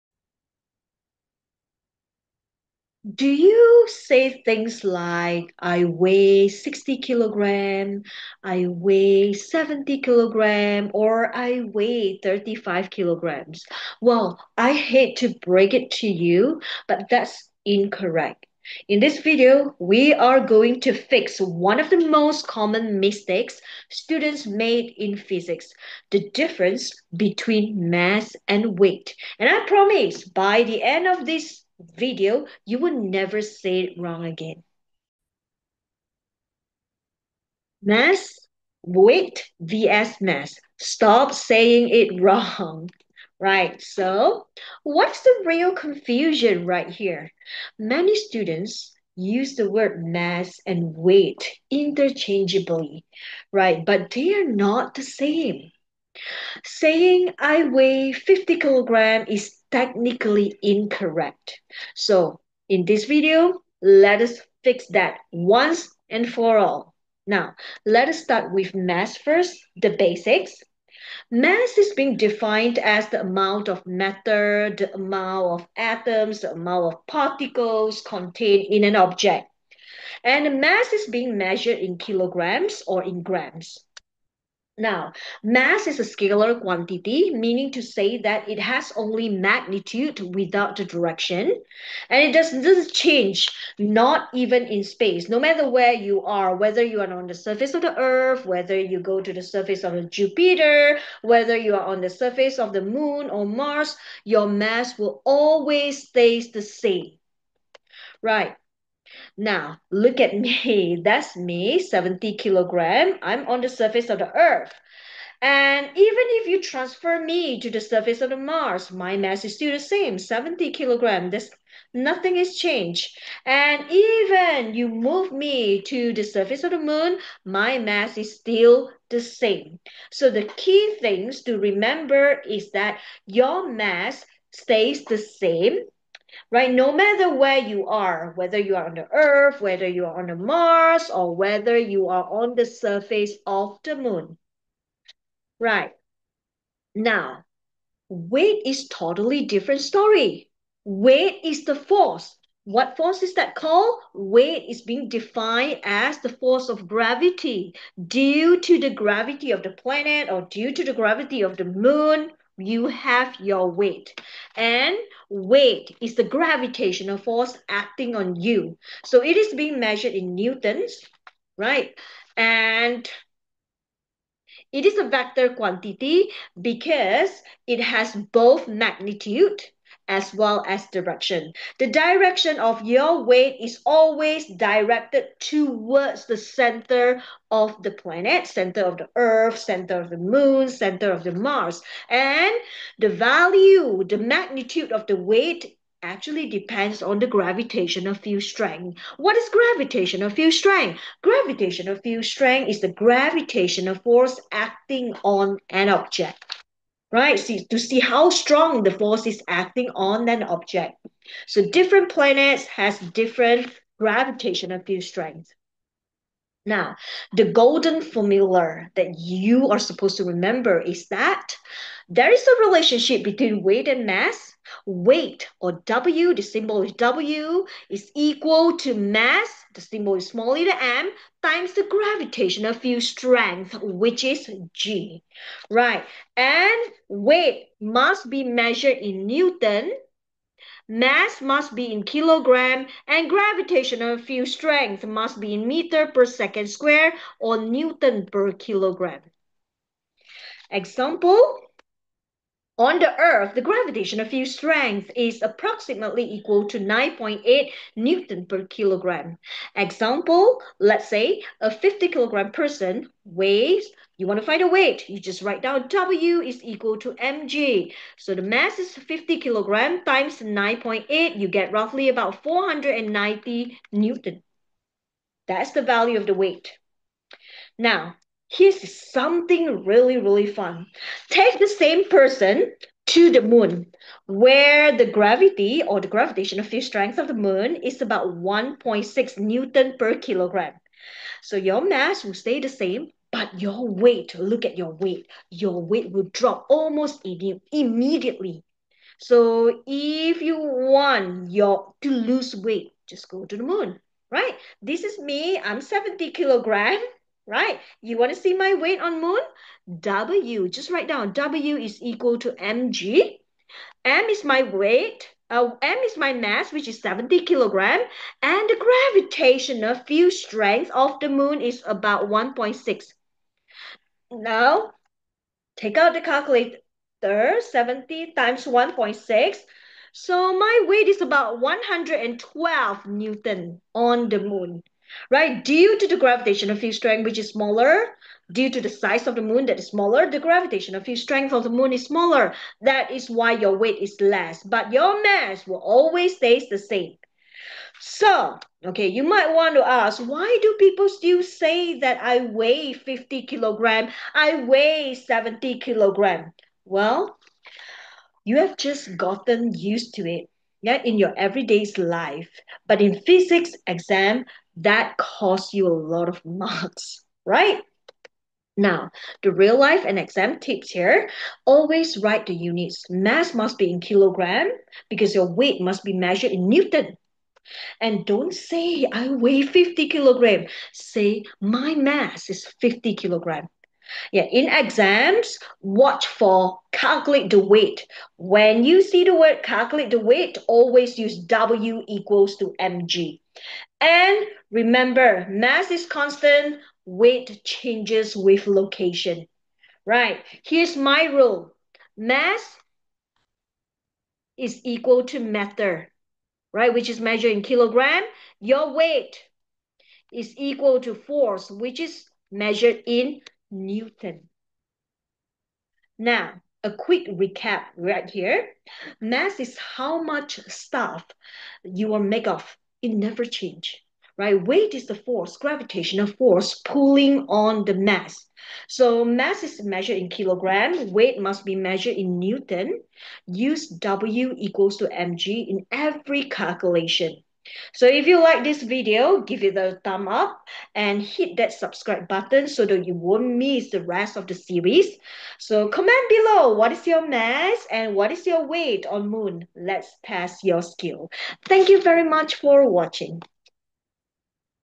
“Weight vs Mass | What’s the Difference? | Physics Explained Simply!”
Weight vs Mass – STOP Saying It Wrong!
Are you still saying "I weigh 50kg"? 😱 Let's clear up one of the most common Physics misconceptions — the difference between weight and mass. In this IGCSE Physics lesson, Tanya Chew breaks it down using simple explanations, real-life examples, and quick exam tips you won’t forget.
🎯 What You’ll Learn:
• The real difference between mass and weight
• Why your mass stays the same but your weight changes
• How to use the formula Weight = Mass × Gravity (W = mg)
💬 Drop your question in the comments or join our Physics study group below 👇